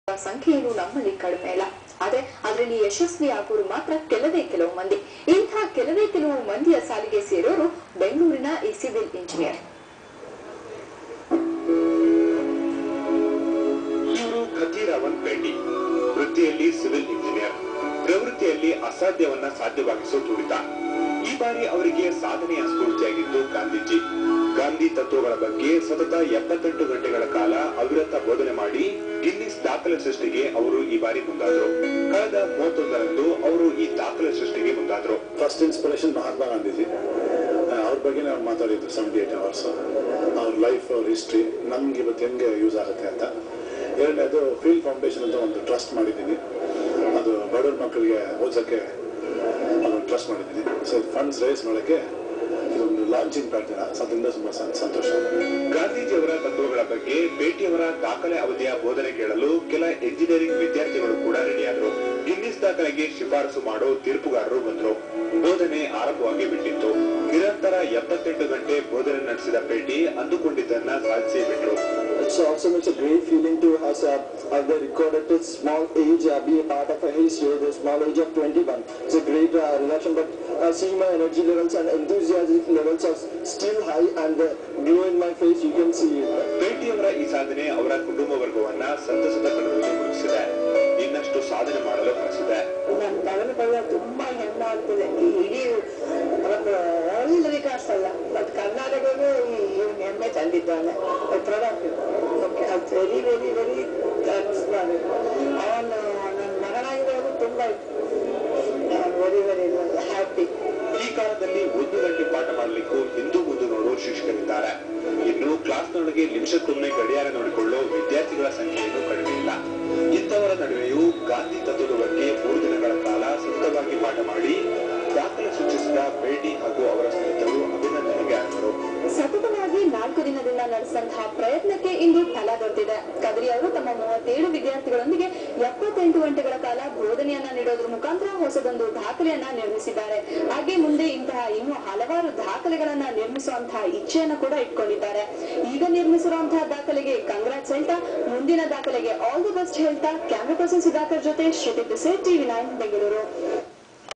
Longest Class Ever...Vijayant Tiwary,Anuj,Ankur
This is the longest class ever has been recorded in guinness book of world records.36 students were taught by Mr. Kathiravan M.Pethi Continuosly for 78 hrs. 3 mnts.